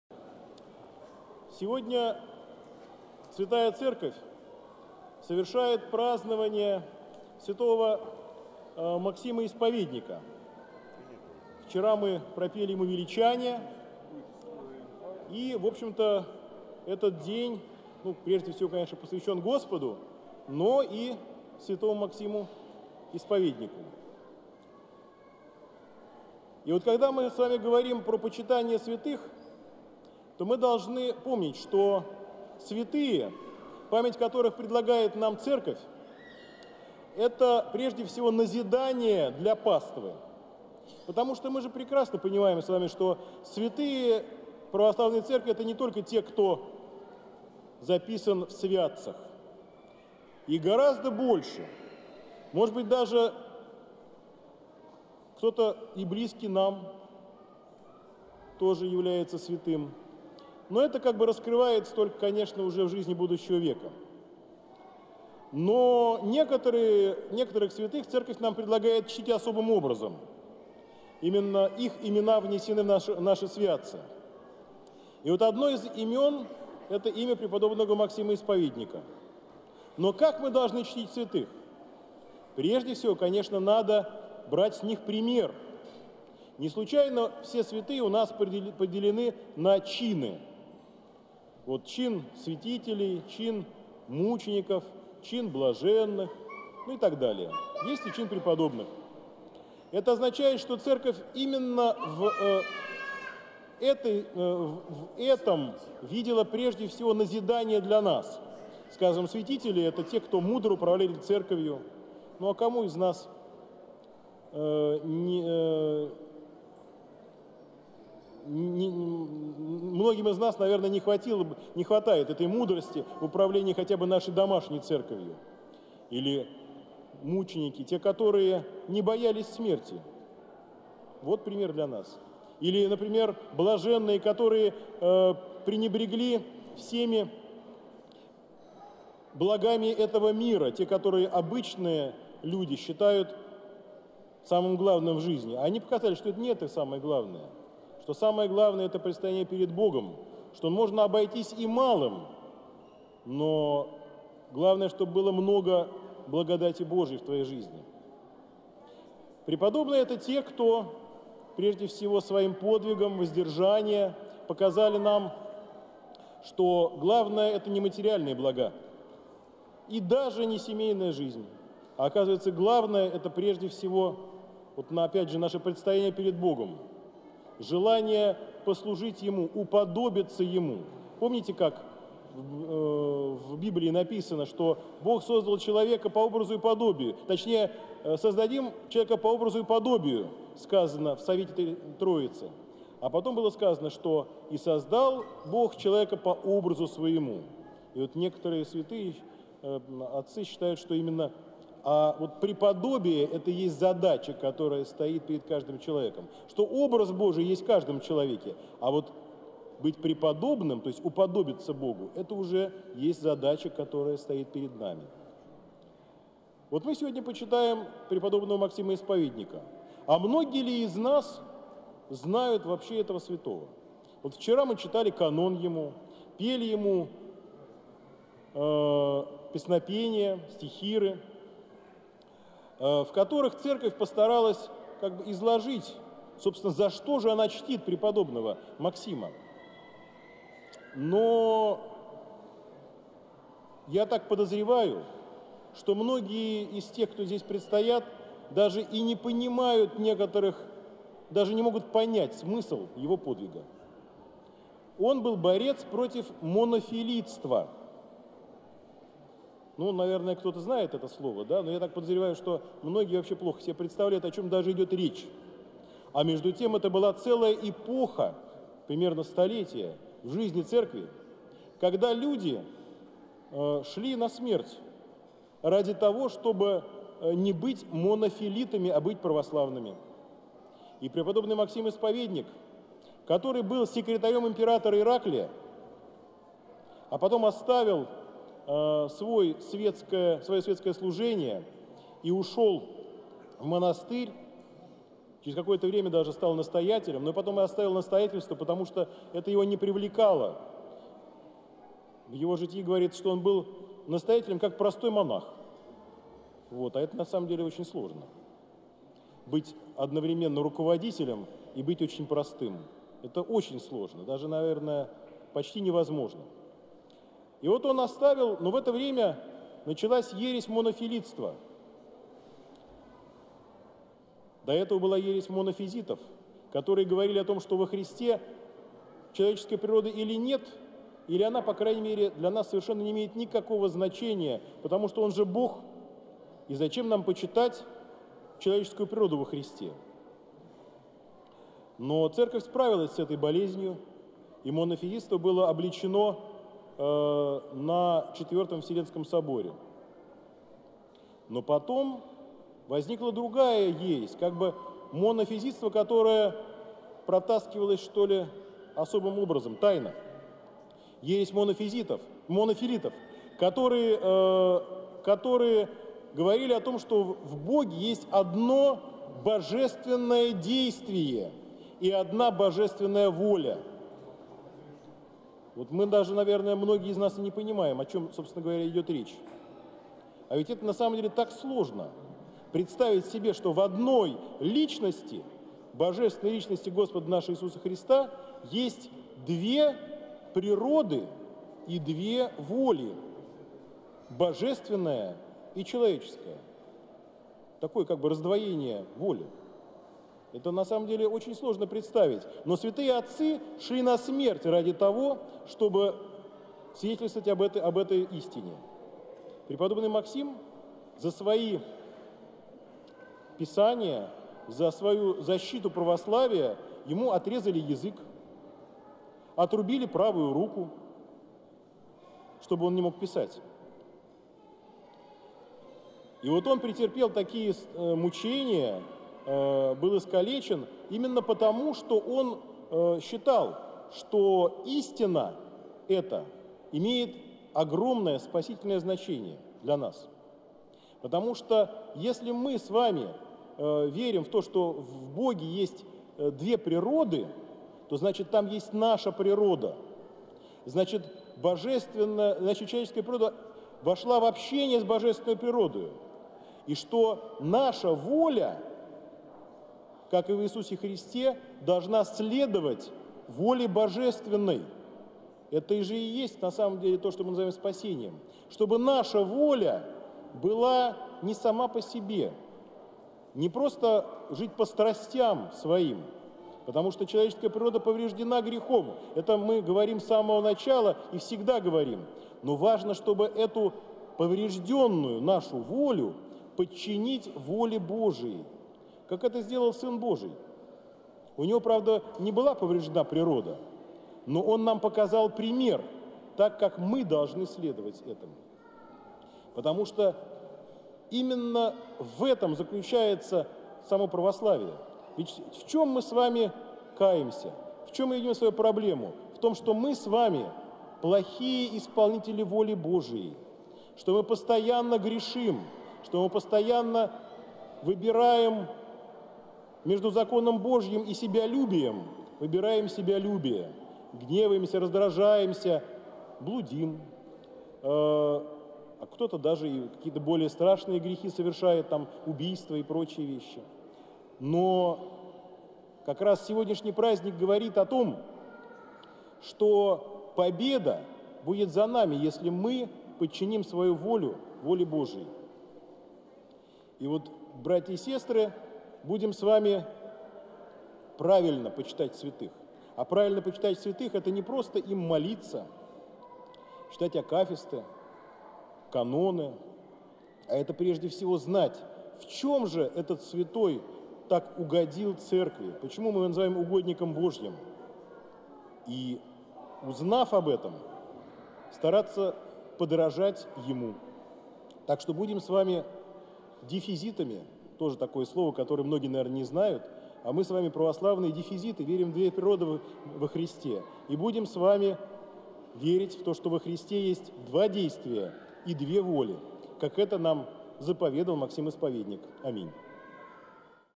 Проповедь 03 02 2019
Отец Дмитрий Лин - воскресная проповедь. Храм Свт. Николая Чудотворца на Трех горах.